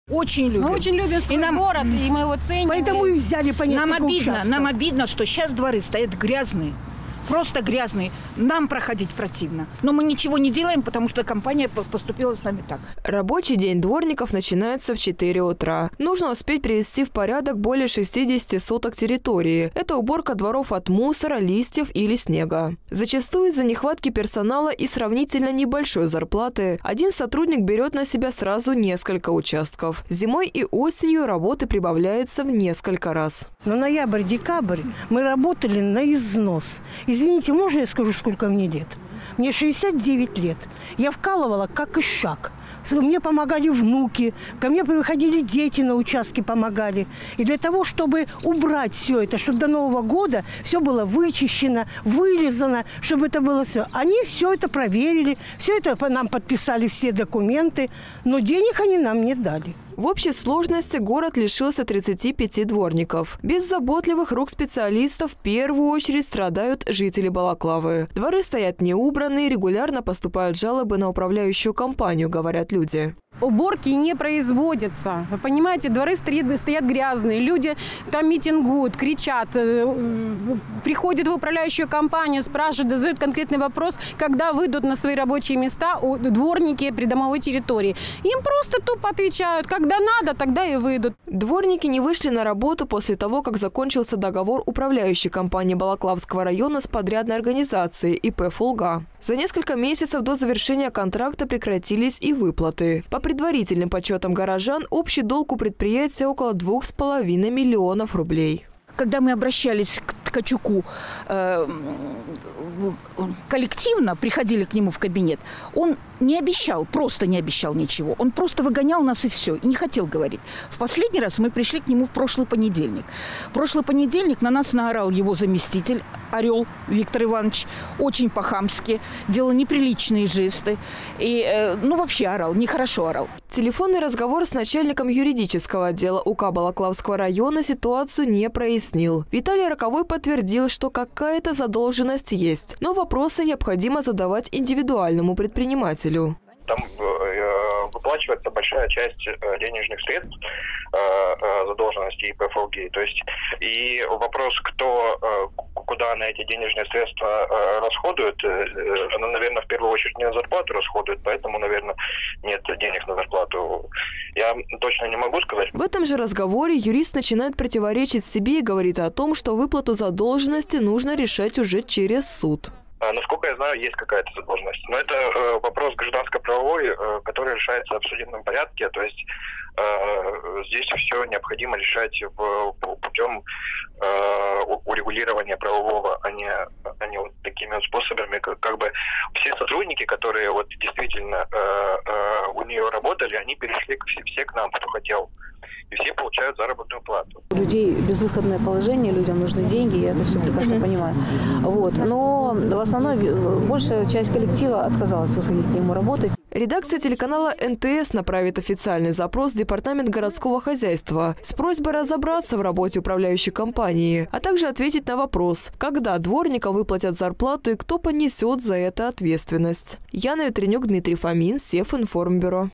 Дворникам Балаклавы несколько месяцев не выплачивают зарплату
Дворникам Балаклавы несколько месяцев не выплачивают зарплату. В редакцию телеканала НТС обратились сотрудники подрядной организации управляющей компании Балаклавского района. По словам людей, долг у предприятия – около 2,5 миллионов рублей, заработанных денег дворники не видели с октября прошлого года.